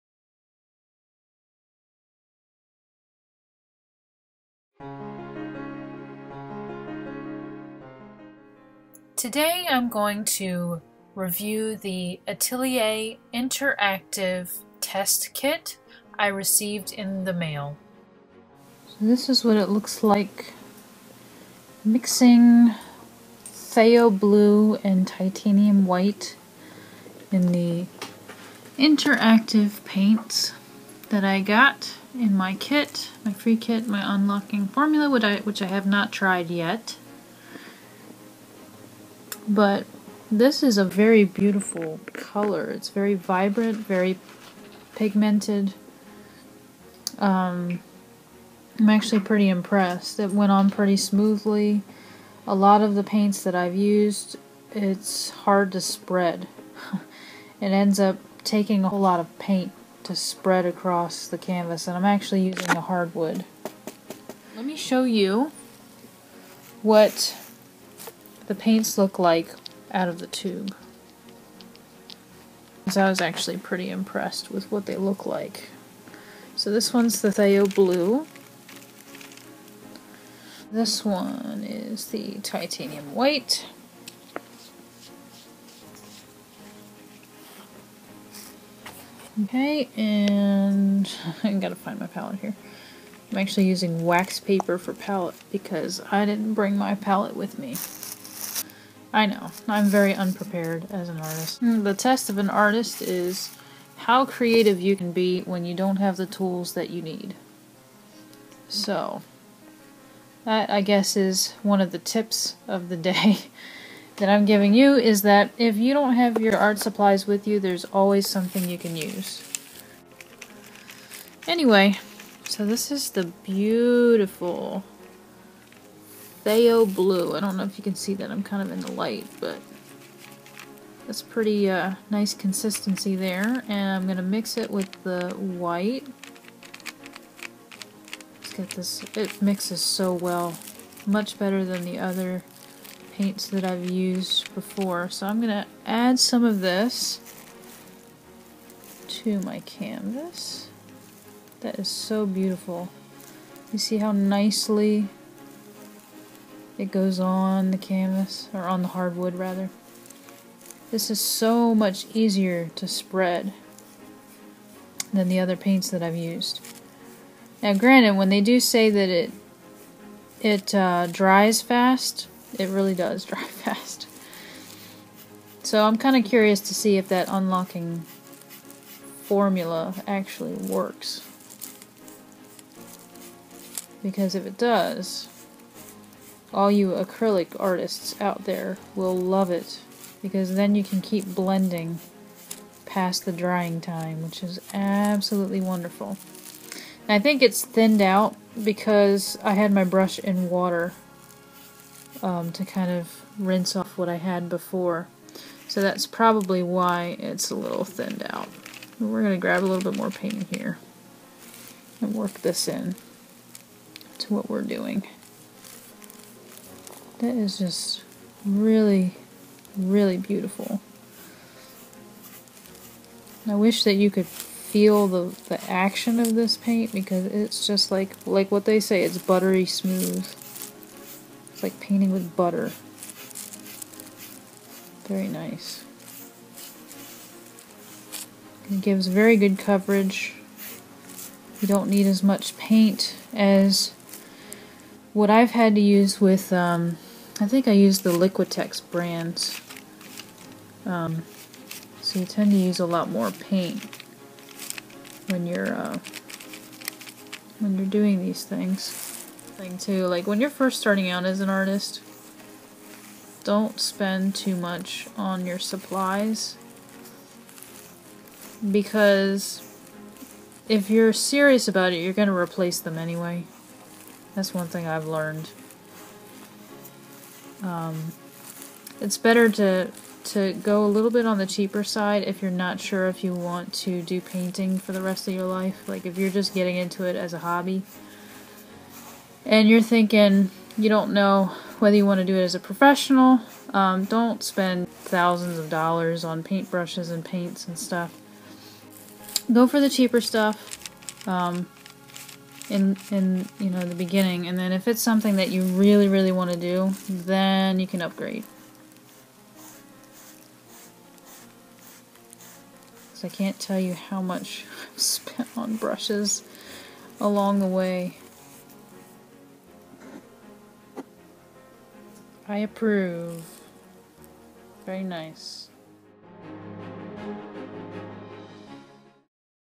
Creator's Image: Atelier Interactive Paint Review
Creator's Image reviews the Atelier Interactive paint kit.